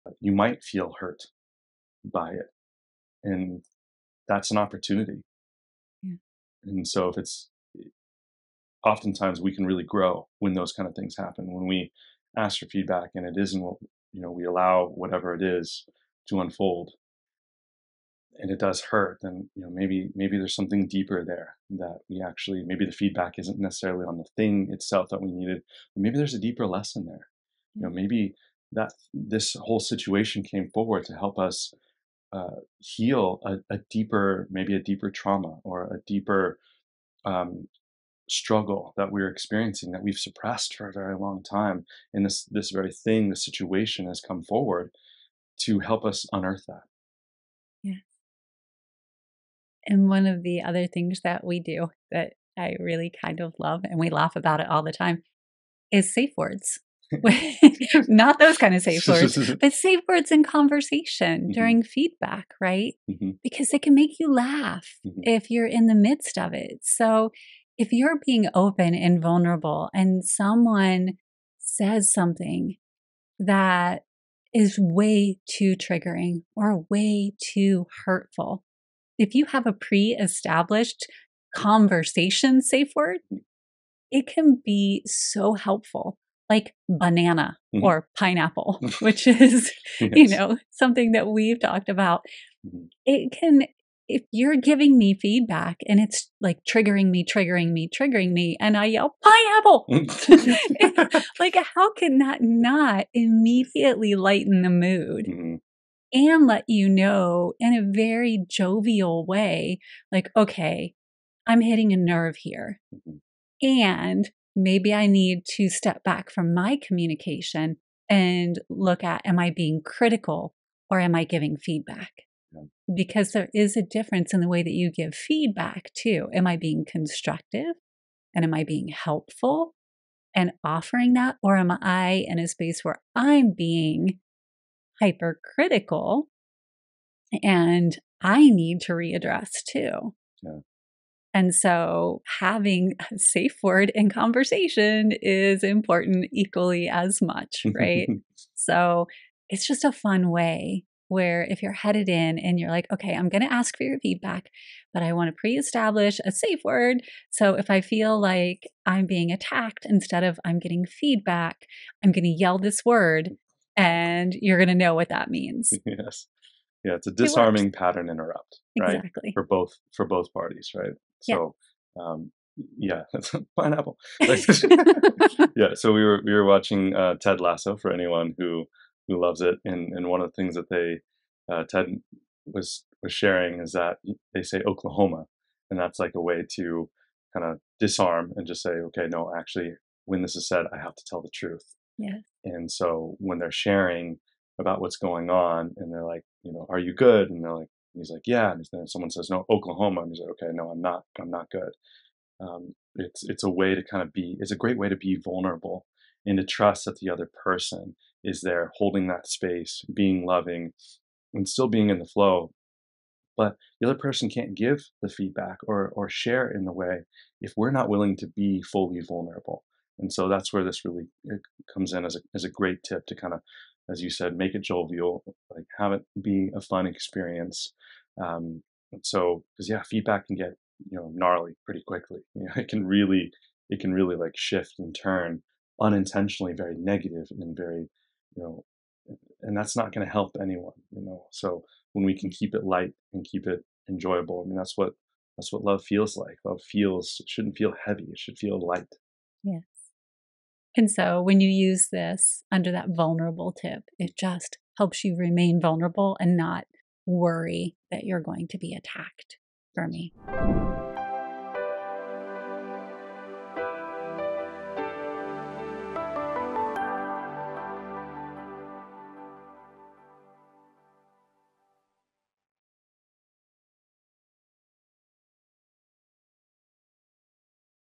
Turning Feedback into Opportunities for Growth | The Heart Leader Podcast
Discover how to transform feedback into growth opportunities with strategies for constructive, light-hearted communication and relationship building.

💬 Join the Conversation: Have you ever used a "safe word" during feedback sessions to keep the conversation light and constructive? Share your experiences and tips in the comments below!

🚀 Join the waitlist for our new transformative community: My New Life Launchpad ➡️

In this insightful episode of The Heart Leader Podcast, we explore how feedback can be a powerful tool for personal growth and healing. Learn how to transform potentially painful feedback into opportunities to unearth deeper issues and foster significant self-improvement. Our discussion includes practical tips on how to make feedback sessions more constructive and less daunting through the use of conversation "safe words" to maintain a light-hearted and supportive environment.

Whether you’re receiving feedback in a professional setting or navigating personal relationships, this episode provides valuable strategies to help you use feedback to its fullest potential, improving communication and deepening trust.

Join us as we dive into mastering the art of feedback, turning challenging conversations into opportunities for personal and relational growth.

⏰ TIMECODES ⏰
00:00 - Introduction to Feedback as a Growth Tool
00:35 - Finding Deeper Lessons in Feedback
01:16 - Using Safe Words to Manage Feedback Sessions
02:02 - Distinguishing Between Criticism and Constructive Feedback
02:43 - Establishing Safety in Feedback Conversations
03:16 - Encouraging Vulnerability and Honesty
03:42 - Keeping Feedback Light and Productive